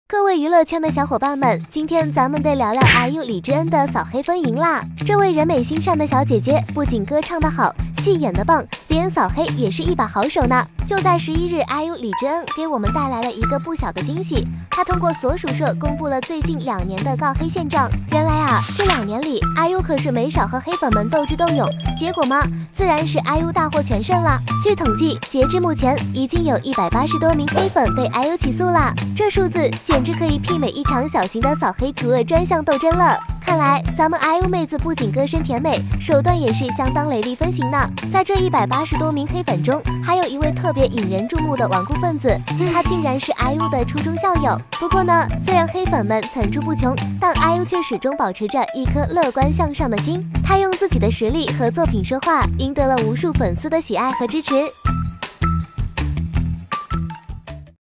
IU李知恩「掃黑行動」大揭秘180名黑粉校友竟成「頑固分子」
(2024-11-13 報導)